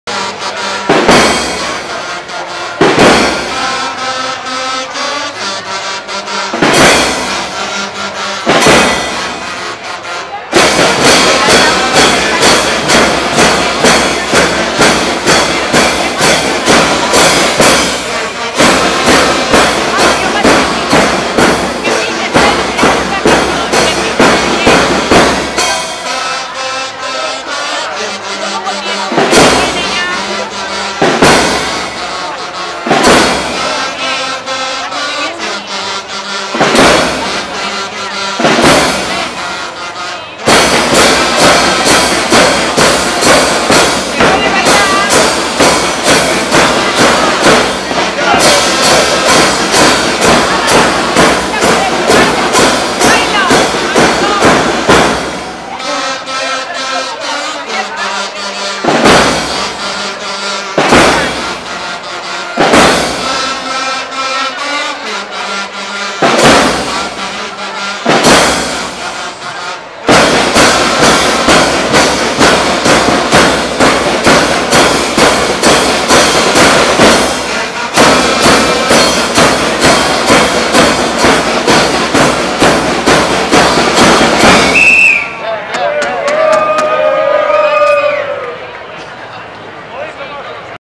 HD Ermua Pasacalles Txaranga Santiagos 2015 Santixauak
Interpretación de una txaranga de las fiestas de Ermua durante la tarde del 26 de julio de 2015, durante el desarrollo de las fiestas patronales de Santiago.